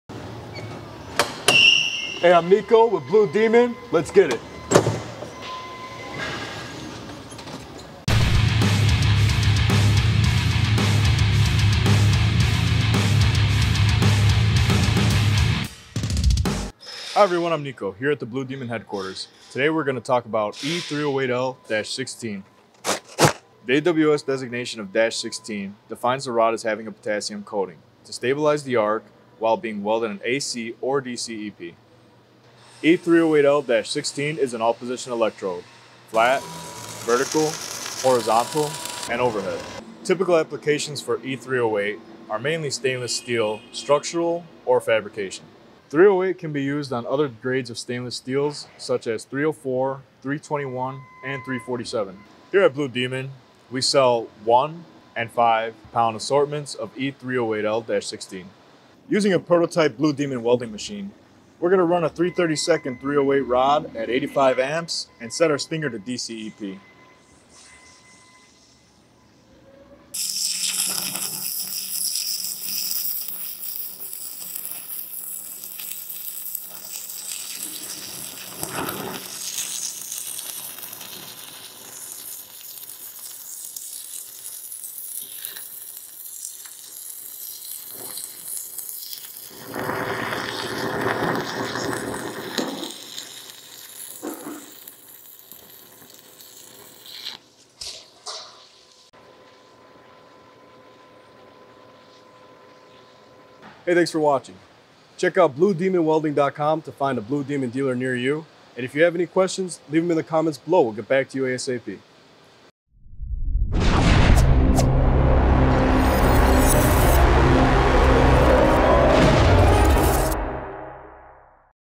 Blue Demon E308/E308L-16 Stainless Steel Covered Arc Welding Electrodes
E308/308L-16 is an extra low carbon electrode for the welding of type 304L, 321, and 347 stainless steels.  The controlled ferrite in the weld deposit gives excellent notch toughness at cryogenic temperatures.  This electrode deposits a max of 4% carbon in the weld metal to minimize the formation of chromium carbides and consequent susceptibility to intergranular corrosion.

AWS Class: E308/308L-16
Certification: AWS A5.4/A5.4M:2006
Alloy: E308L
ASME: SFA A5.4
Welding Position: F, V, OH, H
Current: AC-DCEP
Tensile Strength, kpsi: 75 min
Yield Strength, kpsi: *NS
Elongation %: 35 min

For more information about E308/E308L-16 and to find a Blue Demon dealer near you check out: